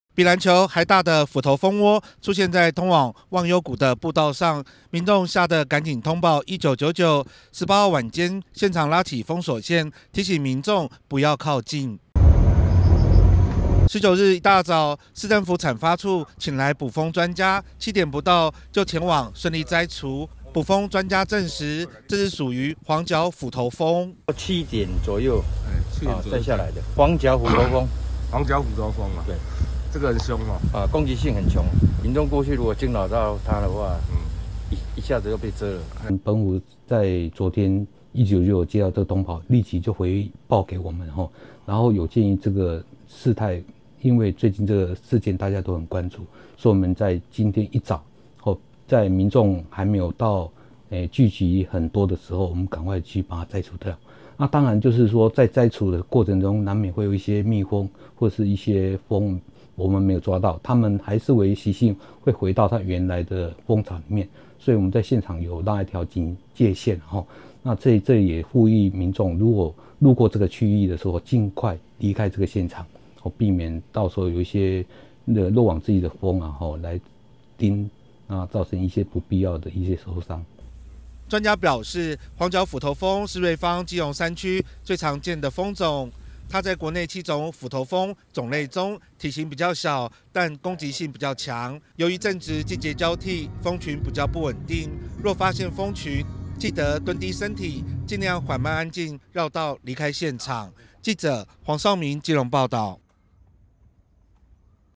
望幽谷｜虎頭蜂巢｜望幽谷步道 赫見虎頭蜂巢「比籃球大」急摘除
(中嘉吉隆新聞 記者黃紹銘 /基隆報導) 有民眾向1999通報，基隆八斗子油庫通往忘幽谷步道，18日發現一顆巨型虎頭蜂巢，市府產發處一早派員前往摘除，蜂巢直徑有40公分，經過捕蜂人員證實為黃腳虎頭蜂，攻擊性非常強，與9月在瑞芳山區造成2死9傷虎頭蜂種類相同，市府提醒民眾，最近季節交替各地頻傳虎頭蜂螯傷民眾事件，希望登山健行，要特別注意。
比籃球還大的虎頭蜂窩，出現在通往望幽谷的步道上，民眾嚇得趕緊通報1999，十八日晚間現場拉起封鎖線，提醒不要靠近。
十九日一早，市政府產發處請來捕蜂專家，七點不到，就前往順利摘除，捕蜂專家證實，屬於黃腳虎頭蜂。
由於外縣市上個月接連發生虎頭蜂攻擊登山隊伍，傳出傷亡消息，擔心未落網的虎頭蜂，會回到蜂巢地點，市政府產發處呼籲，民眾盡量避免前往鄰近區域。
黃腳虎頭蜂是瑞芳、基隆山區常見的蜂種，黃腳虎頭蜂在國內7種虎頭蜂種類中，體型比較小，但為攻擊性強的蜂種，市府產發處也呼籲民眾，正值季節交替，蜂群比較不穩定，若發現蜂群，記得蹲低身體，盡量緩慢安靜繞道離開現場。